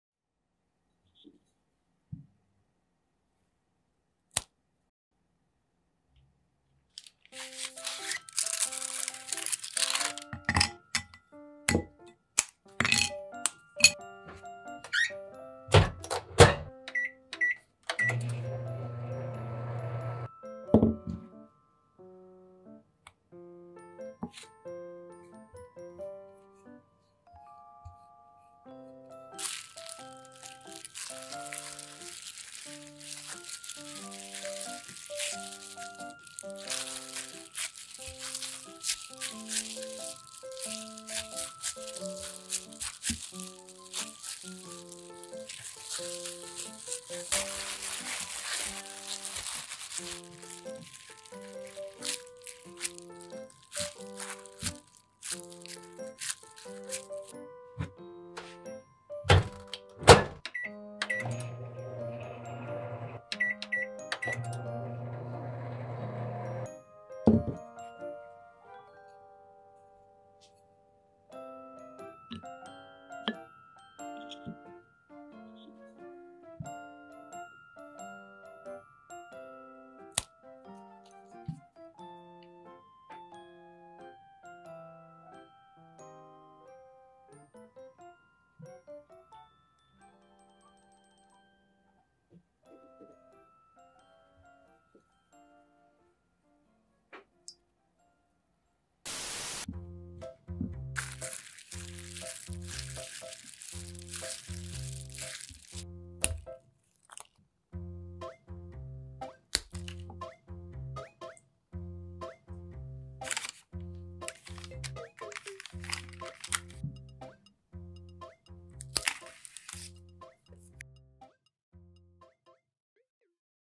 초콜렛으로 쿠키 만들기 :: 초콜렛, 밀가루 2가지 재료면 OK
정말 쉽게 타니까 전자레인지에 돌리실때는 짧게 여러번 돌려주세요~!

재료)
초콜릿, 박력분 밀가루